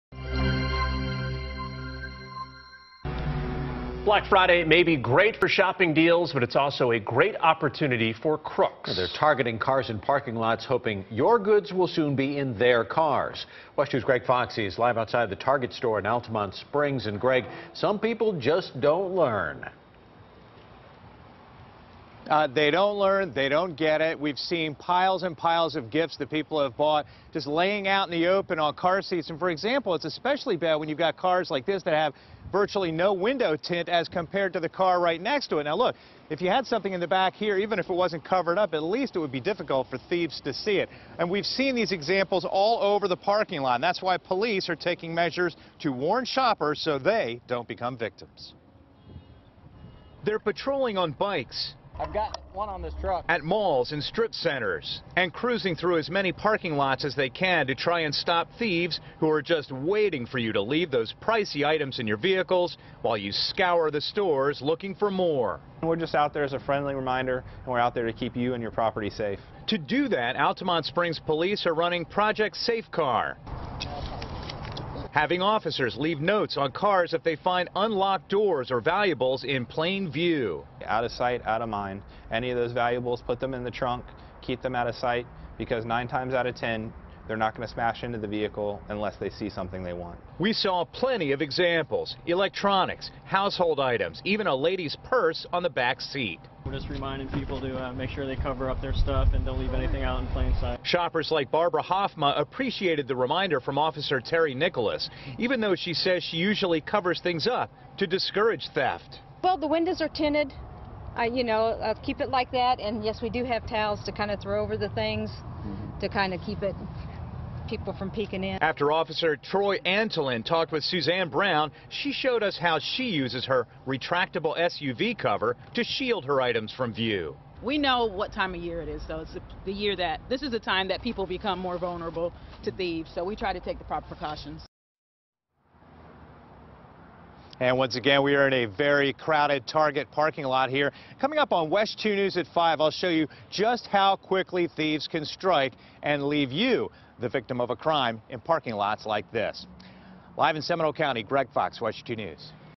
Police: Don't leave Black Friday purchases in plain site
Police are spending Black Friday reminding shoppers to not leave their belongings and purchases in plain site for possible criminals.